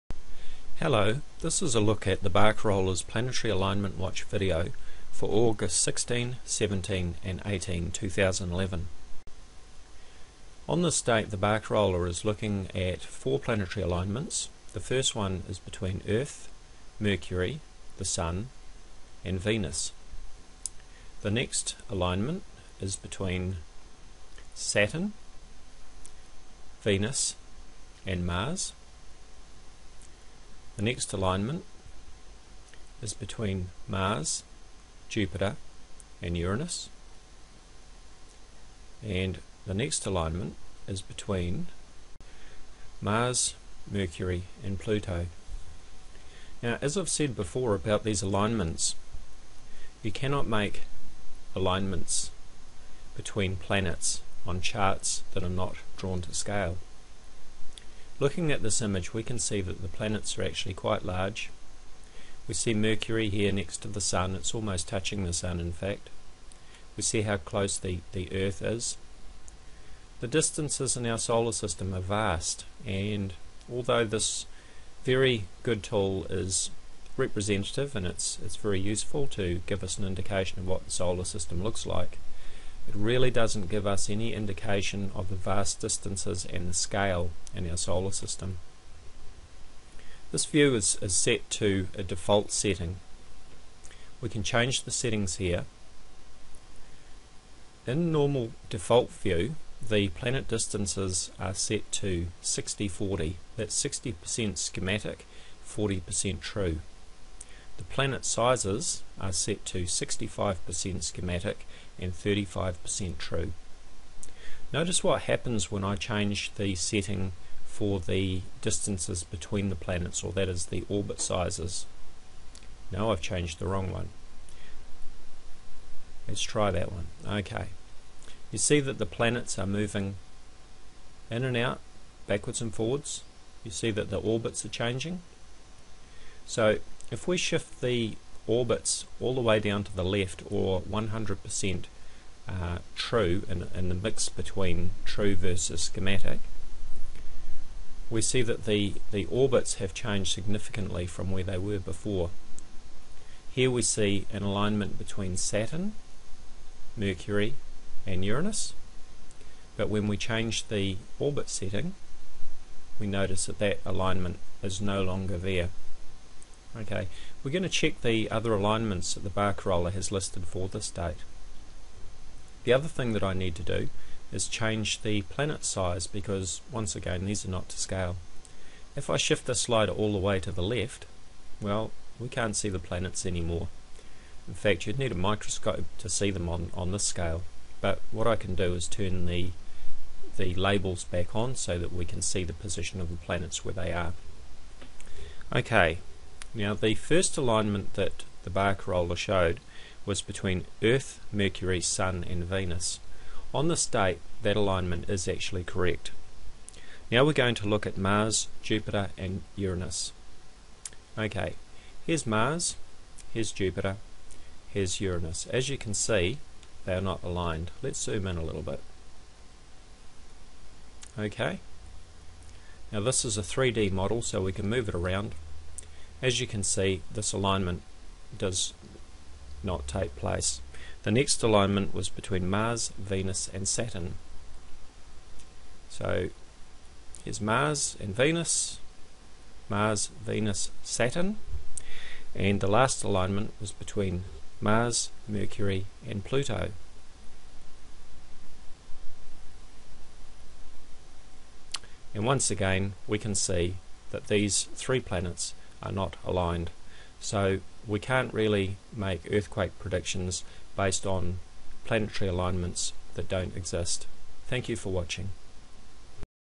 Why Thebarcaroller's Charts Are Wrong - 4
We take a closer look at Thebarcarollers Planetary Alignment Watch video for 16-17-18 August 2011

The free online program used in this video is available at the Solar System Scope Website